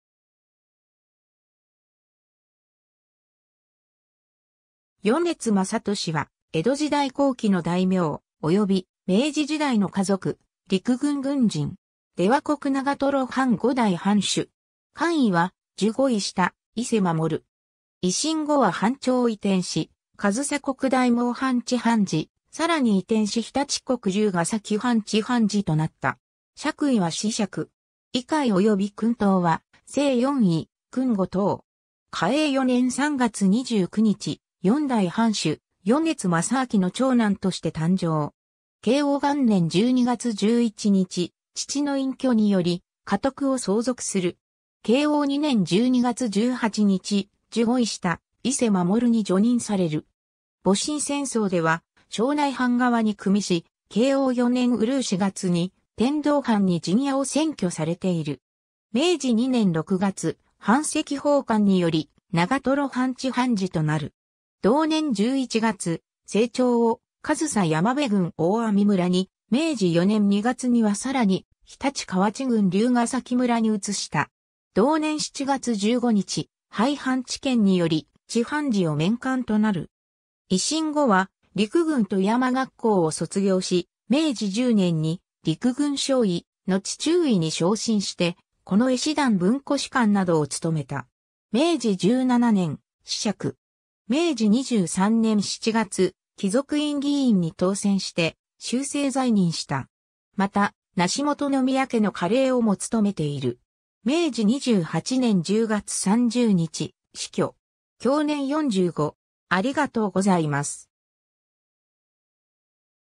米津政敏
米津 政敏（よねきつ まさとし）は、江戸時代後期の大名、および明治時代の華族、陸軍軍人。
出羽国長瀞藩5代藩主。
官位は従五位下・伊勢守。
維新後は藩庁を移転し、上総国大網藩知藩事、さらに移転し常陸国龍ヶ崎藩知藩事となった。
爵位は子爵。
位階および勲等は正四位・勲五等。
 嘉永4年（1851年）3月29日、4代藩主・米津政明の長男として誕生。
慶応元年（1865年）12月11日、父の隠居により家督を相続する。
 慶応2年（1866年）12月18日、従五位下・伊勢守に叙任される。
戊辰戦争では庄内藩側に与し、慶応4年（1868年）閏4月に天童藩に陣屋を占拠されている。
明治2年（1869年）6月、版籍奉還により長瀞藩知藩事となる。
同年11月、政庁を上総山辺郡大網村に、明治4年（1871年）2月にはさらに常陸河内郡龍ヶ崎村に移した。
同年7月15日、廃藩置県により、知藩事を免官となる。
 維新後は陸軍戸山学校を卒業し、明治10年（1877年）に陸軍少尉、のち中尉に昇進して近衛師団文庫主管などを務めた。
明治17年（1884年）、子爵。
明治23年（1890年）7月、貴族院議員に当選して終生在任した。
また、梨本宮家の家令をも勤めている。
 明治28年（1895年）10月30日、死去。
享年45。